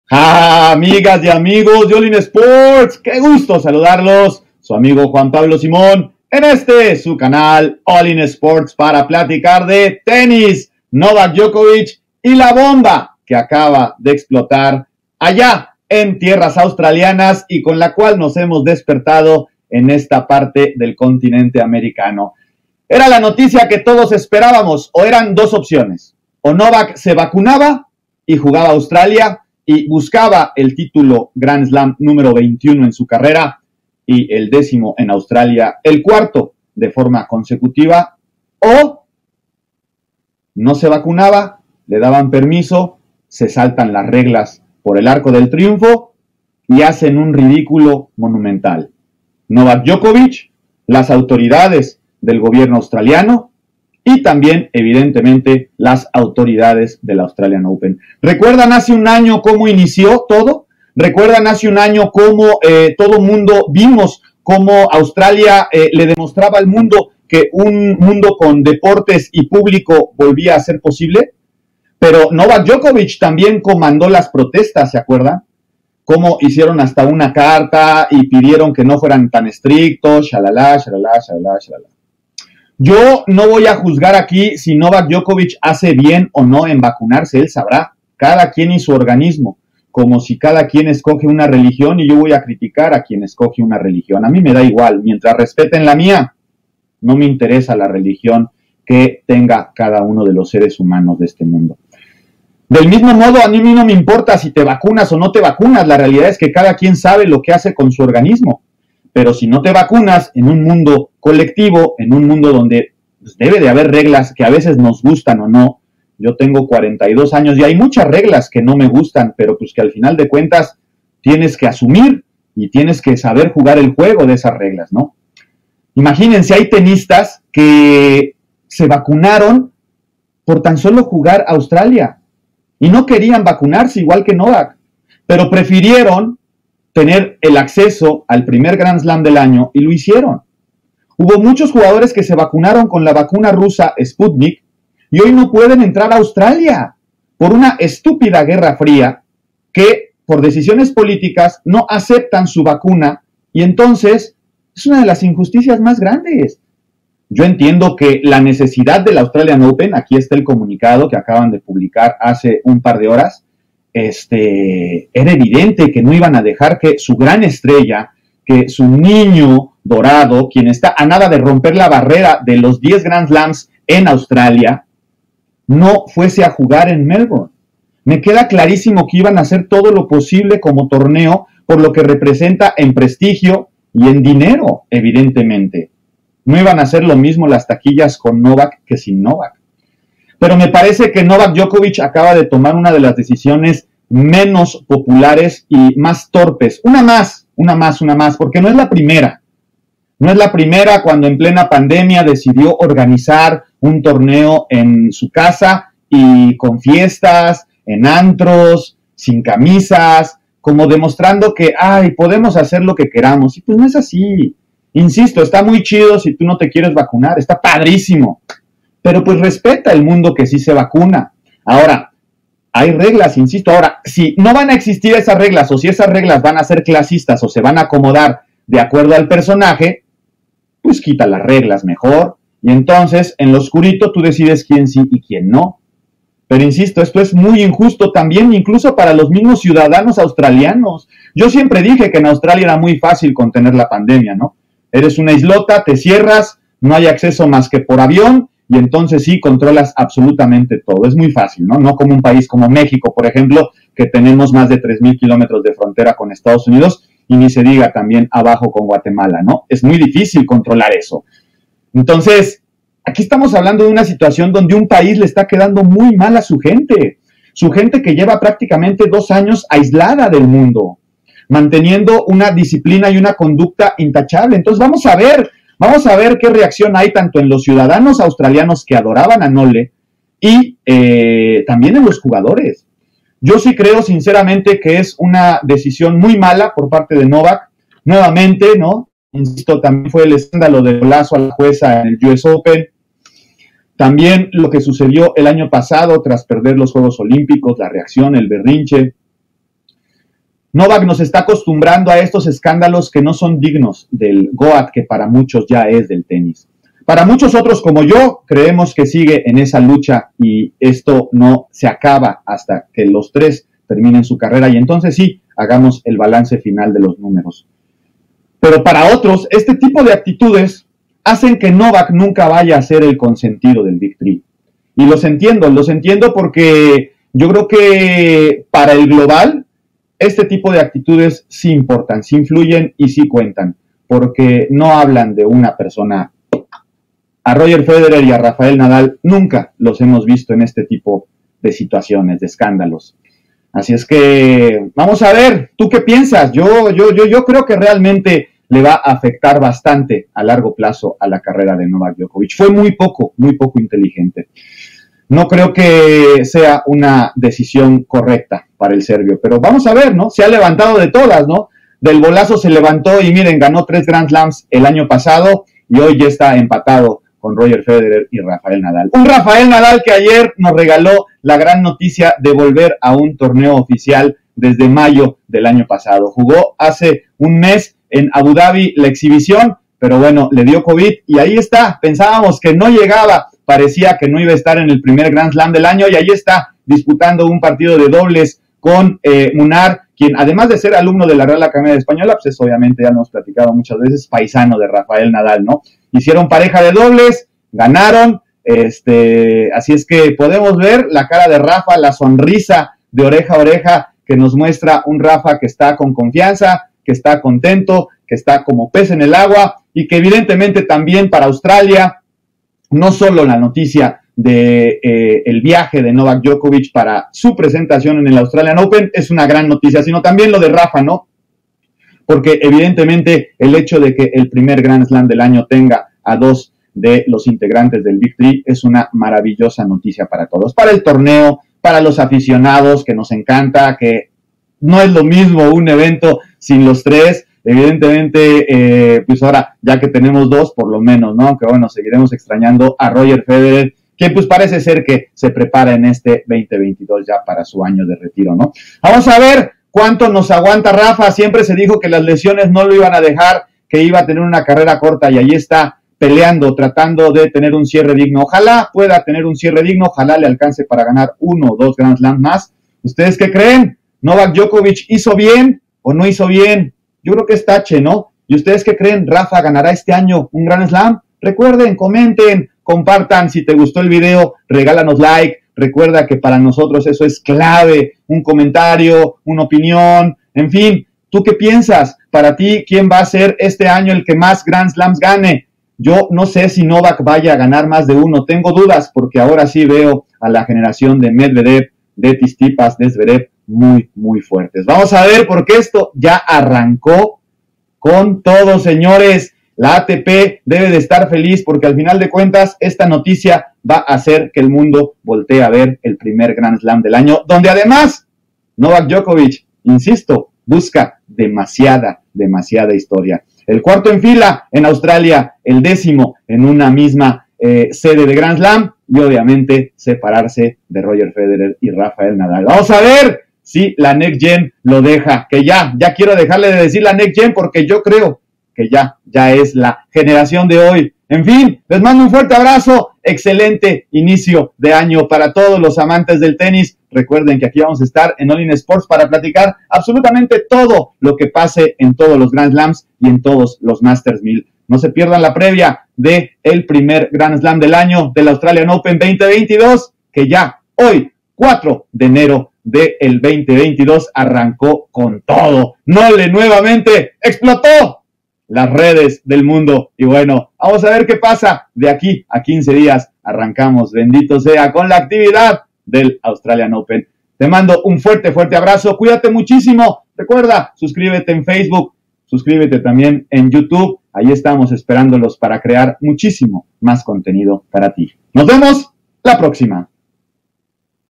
DJOKOVIC SÍ JUGARÁ EL AUSTRALIAN OPEN CON UN PERMISO ESPECIAL Y DESATA POLÉMICA MUNDIAL 💥🎾
El tenista serbio recibió una exención por parte del Gobierno Australiano y podrá buscar el Gran Slam número 21, el décimo en tierras australianas, el cuarto de forma consecutiva, Novak tenía que estar en Australia, la historia así lo necesita, pero no así…